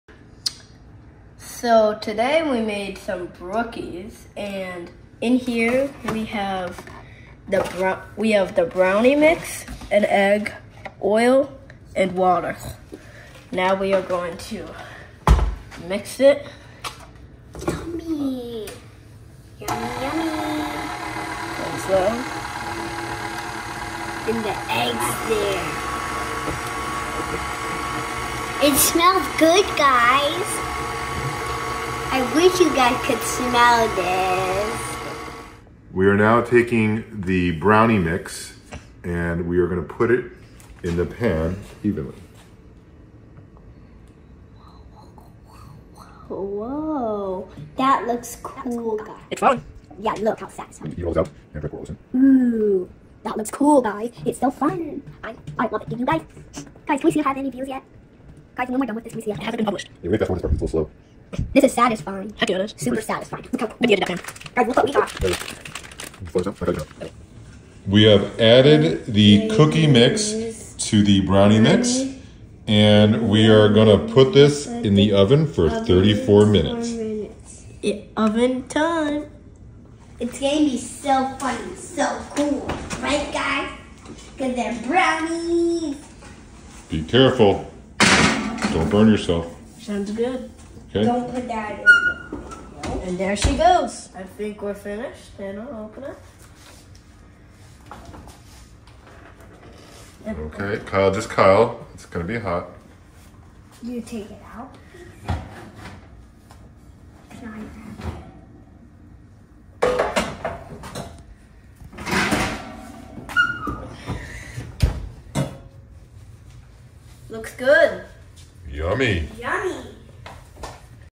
Baking Brookies - Day 2467
On Day 2467 of the countdown, I bake brookies with the kids!